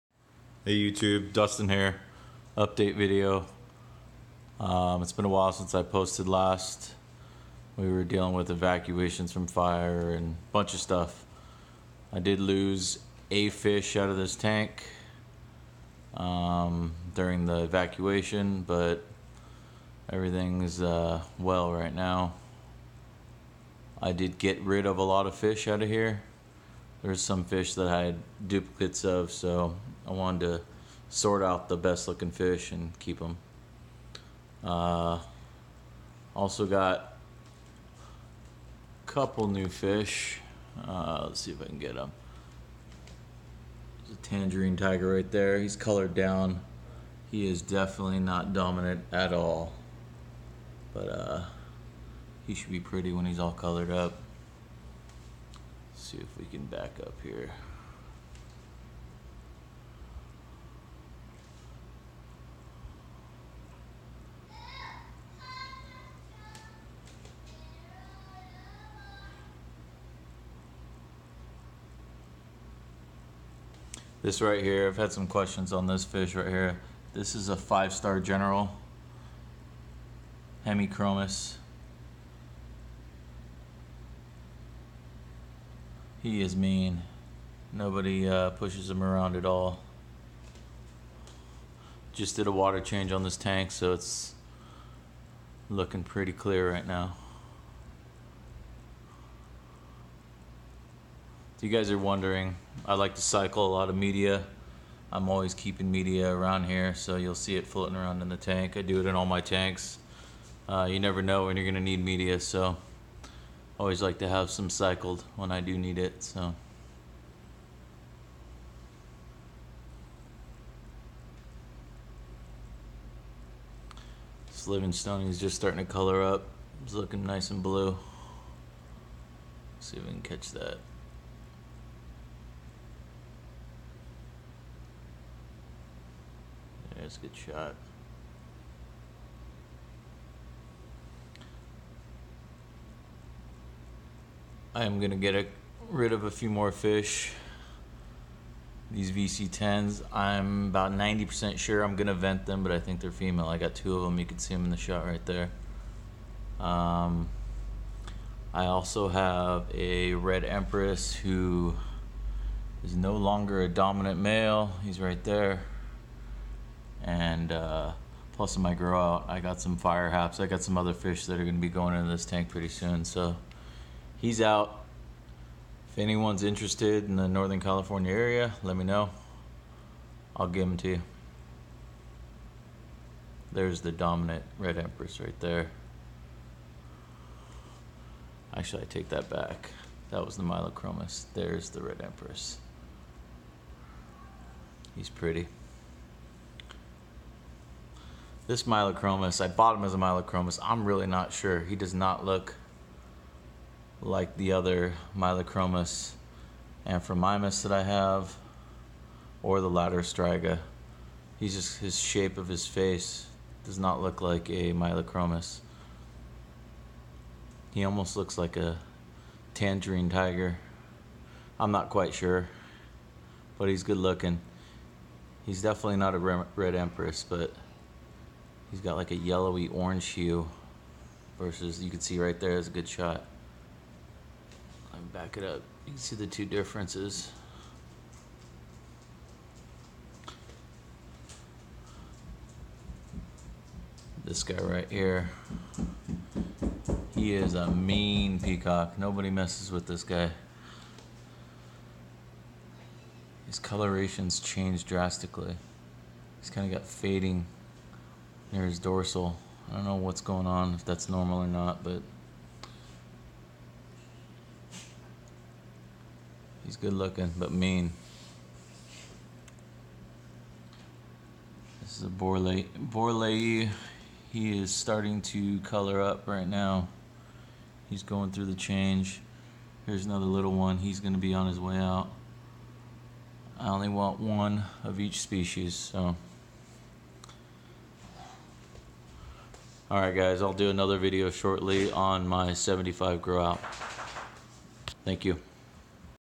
Water Change in the 120 Gallon Hap/Peacock Show Tank
It's been a while since my last update. We were evacuated due to a massive fire near our neighborhood. I lost one fish (Polystigma) and have gotten rid of several others since my last video.